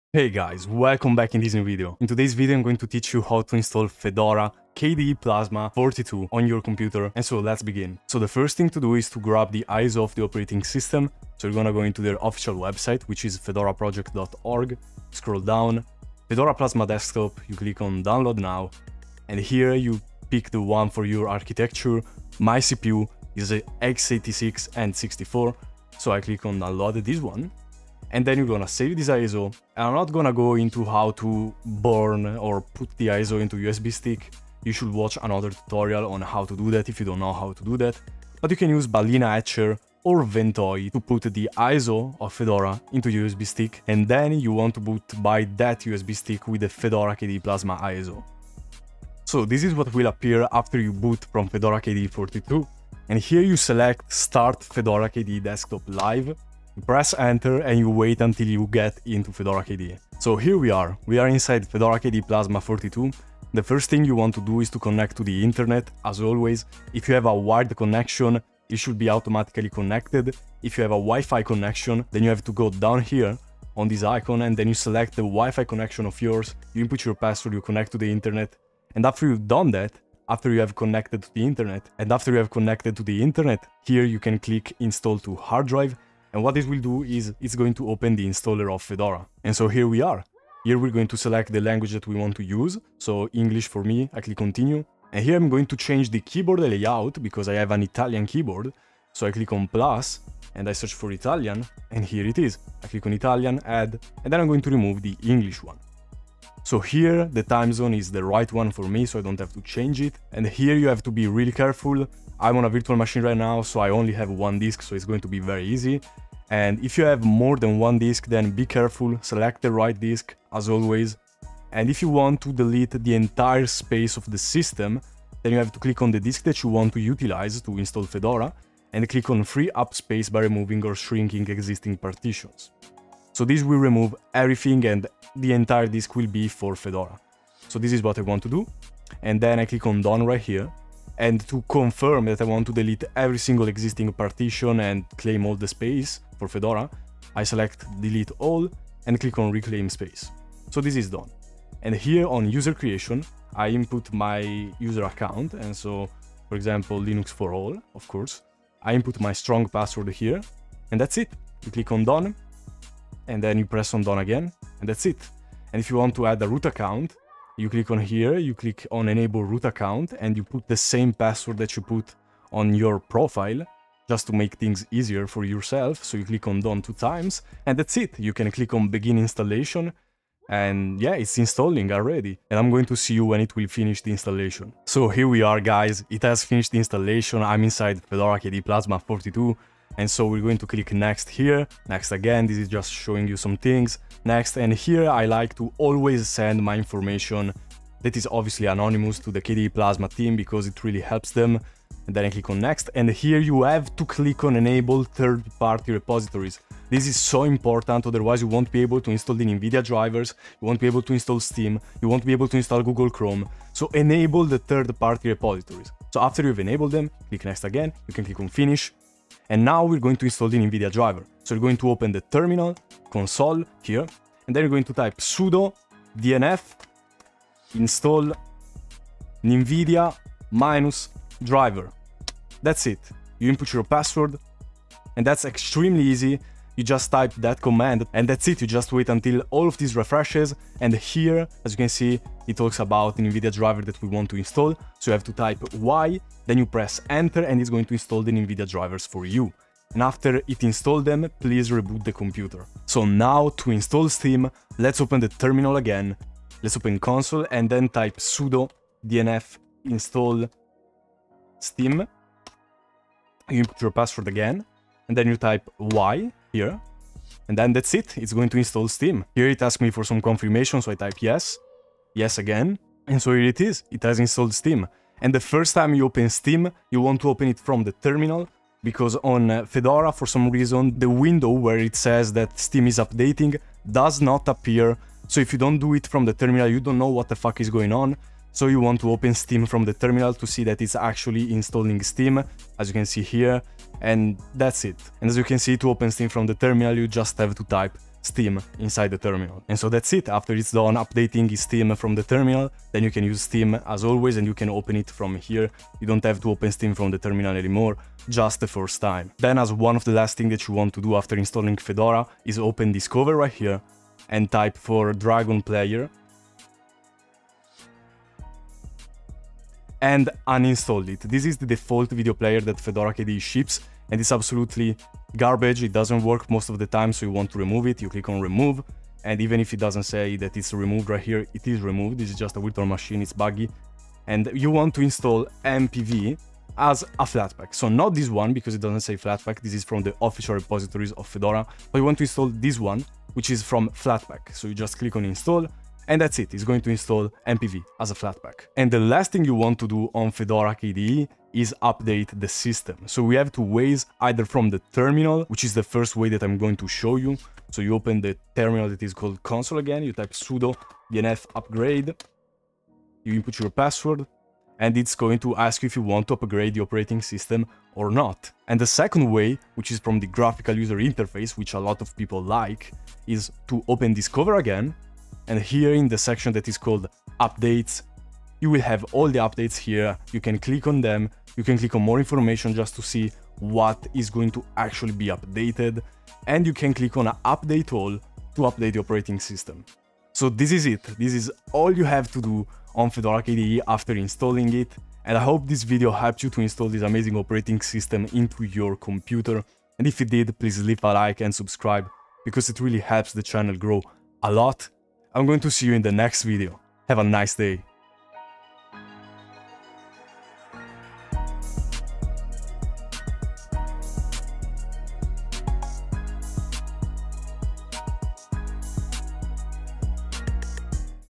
HOW TO INSTALL FEDORA KDE PLASMA 42!
Hello everyone! in this video I'm gonna show you how to install Fedora KDE Plasma 42, and it's super easy, enjoy!